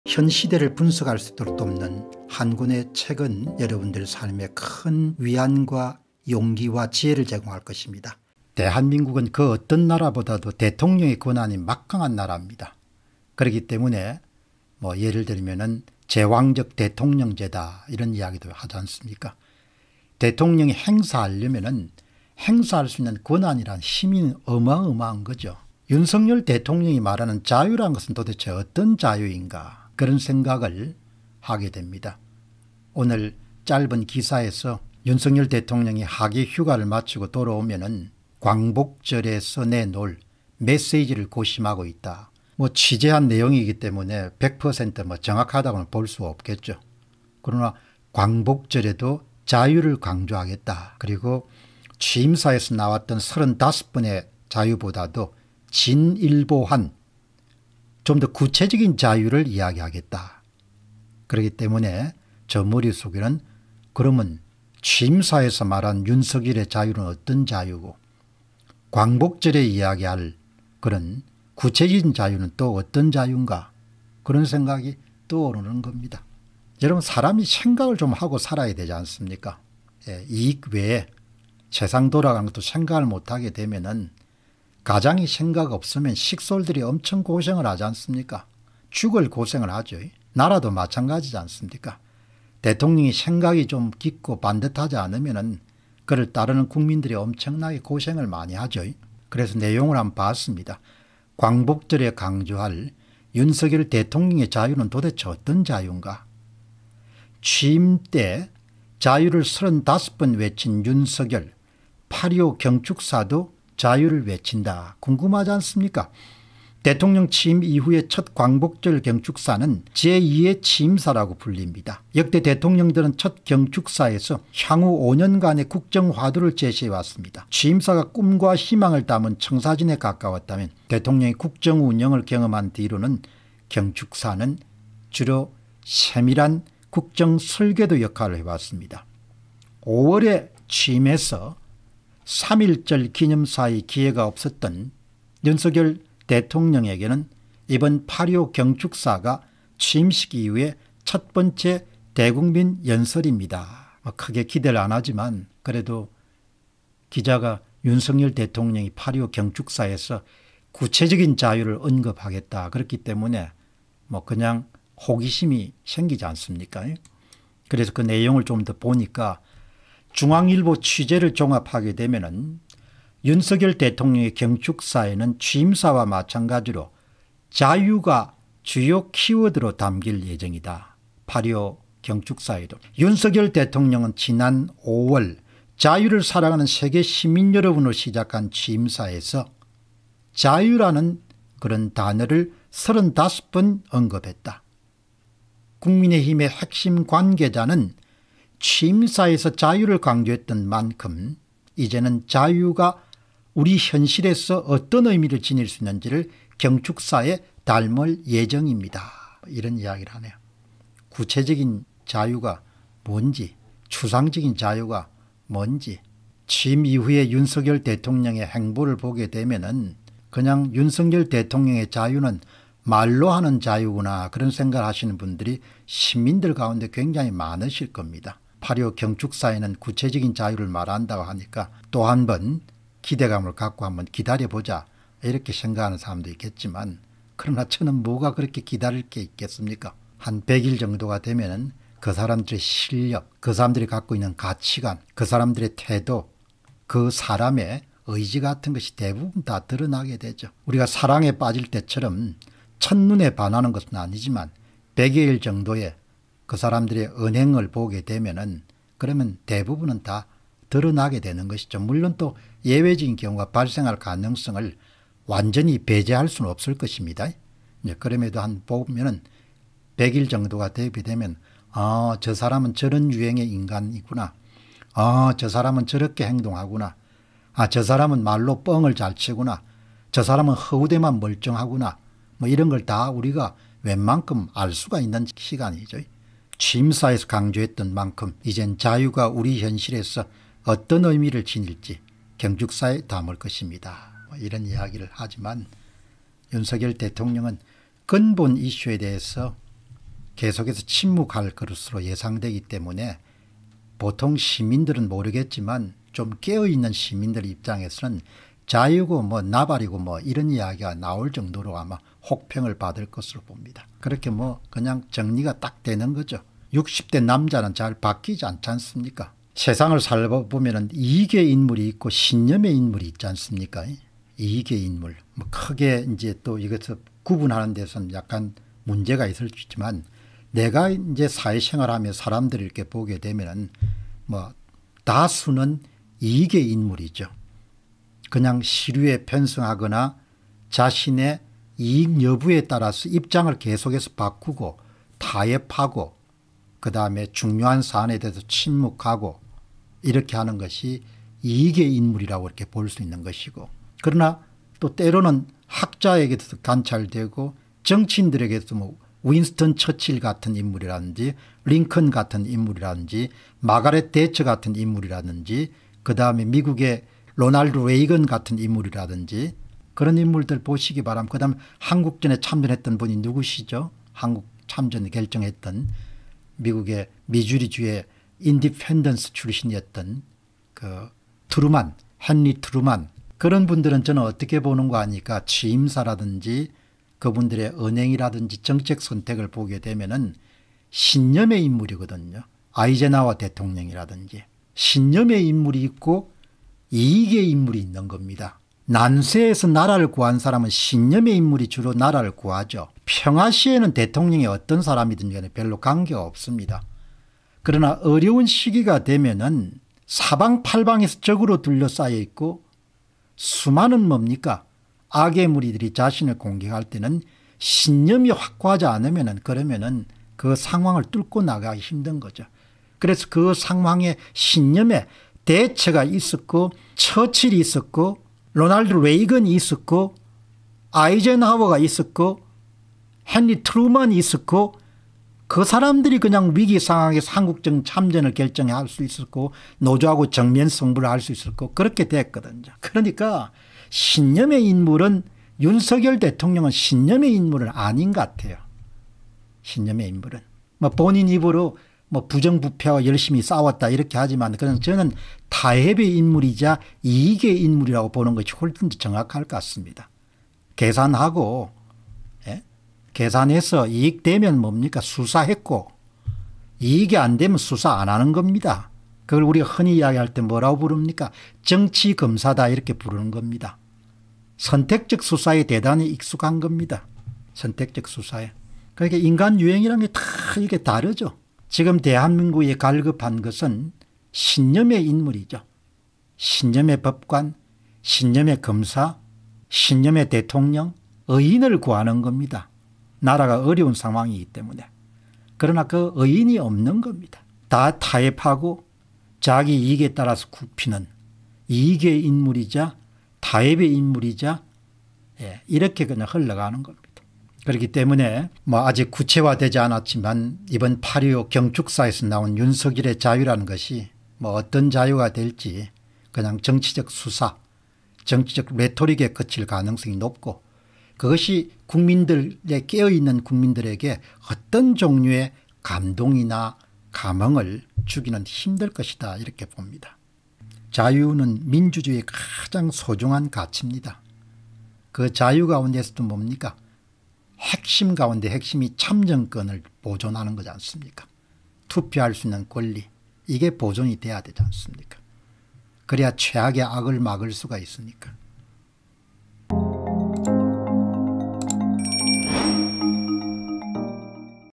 윤석열의 자유 "광복절의 자유, 한참 궁금" / 자유는 민주주의의 가장 소중한 가치 [공병호TV]
Bitcoin : 3MTW849AkP2ftLeSK2teZHMmzLu7Z83s9U